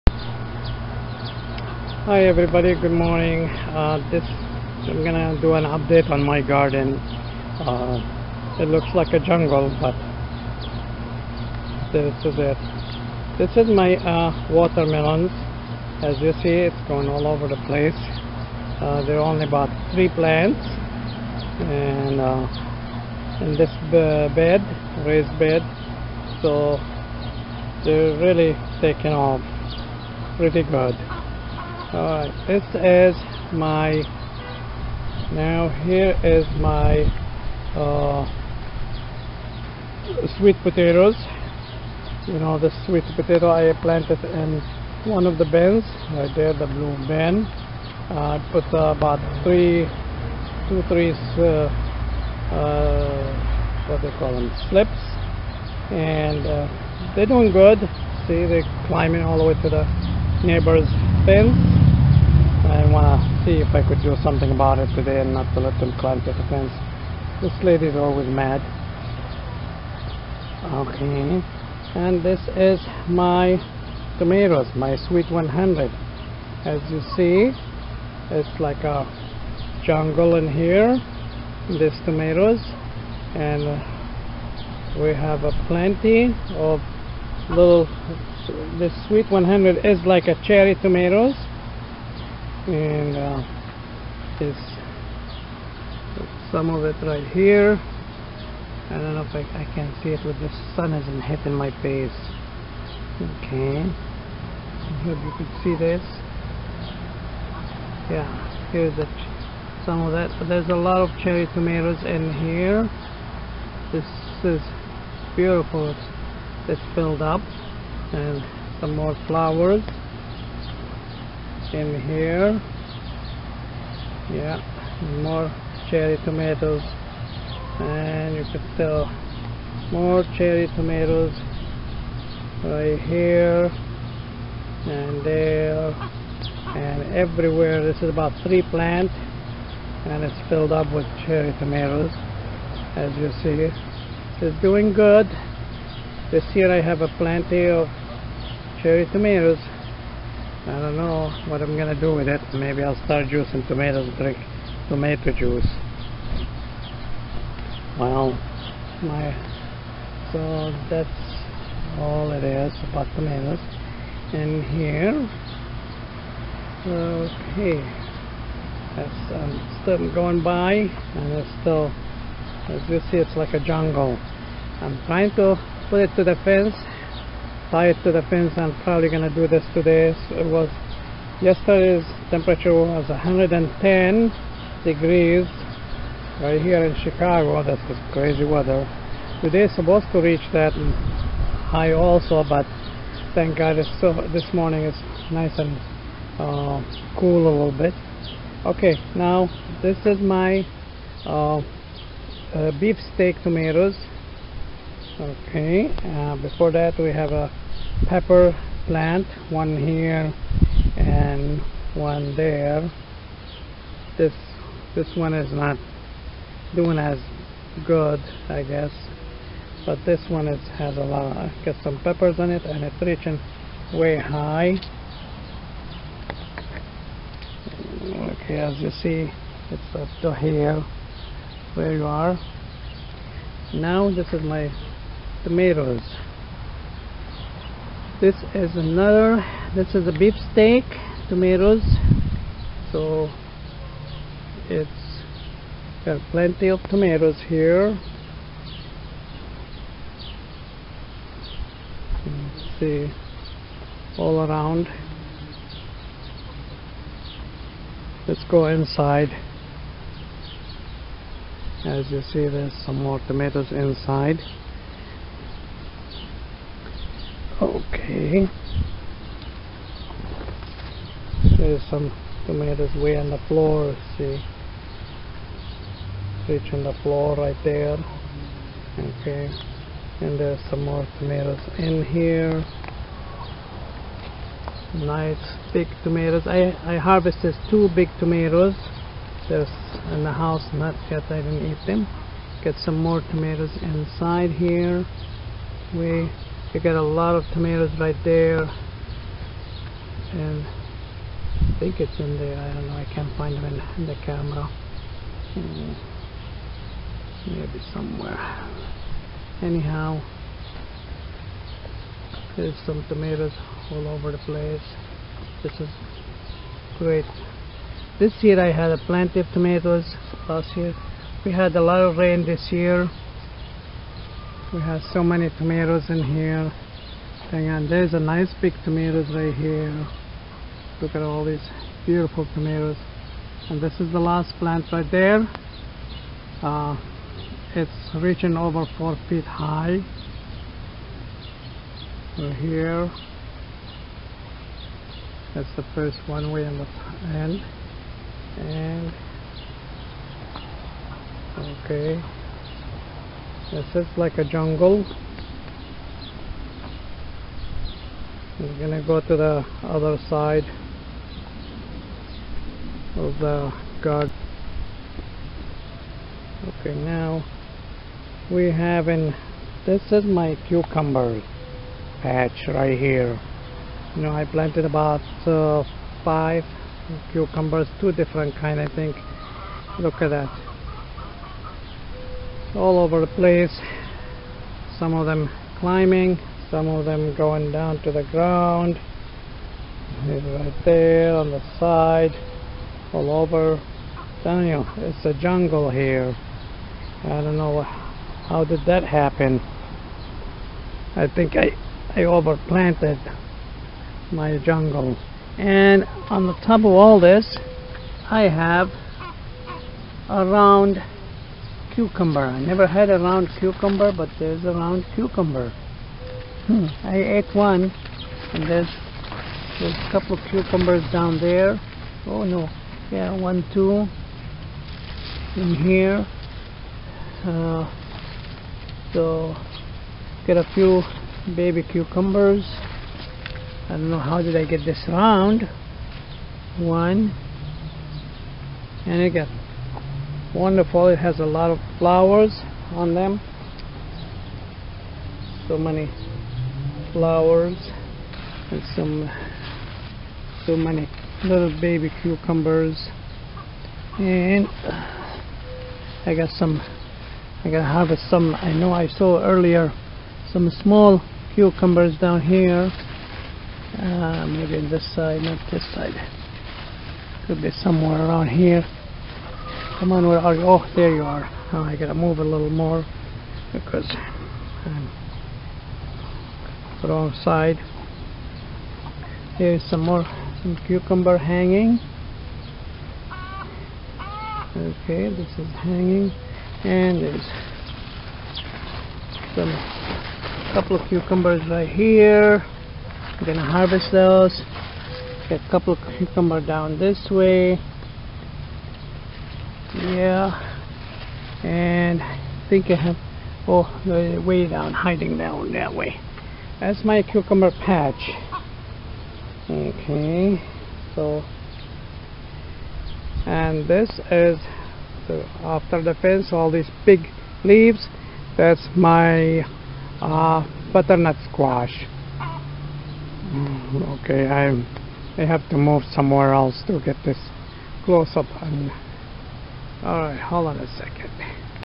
Water Melon cherry tomatoes beef steak tomatoes cucumbers
What I plant this year
I planted this year watermelon as you see they are doing very good.  Before I plant them I put a hand full of worm casting under each one and I feed them fish fertilizer every time I water them.
I also planted sweet 100 [cherry tomatoes], beefsteak tomatoes and cucumbers and more.